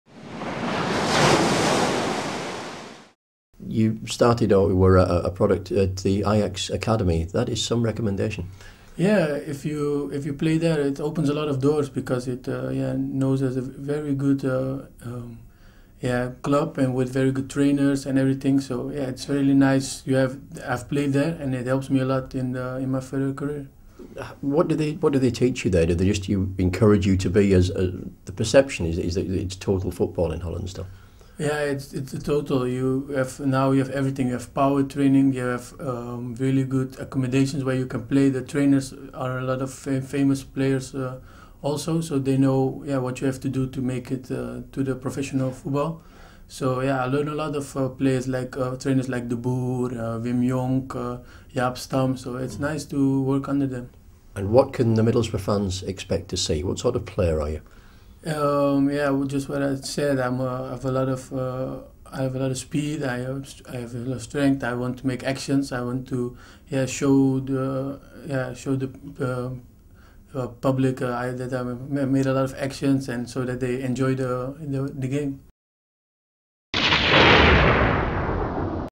Introducing Yanic Wildschut
Boro's latest recruit is a flying Dutch winger who likes to play on the left. For the full, exclusive, interview go to Boro+.